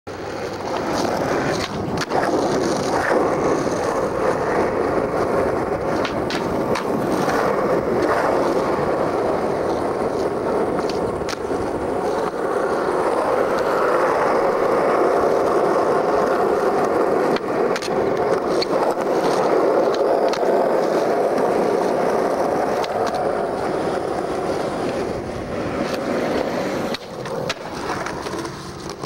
Start it off with a switch frontside shuvit
having fun with this little line at the lawndale church..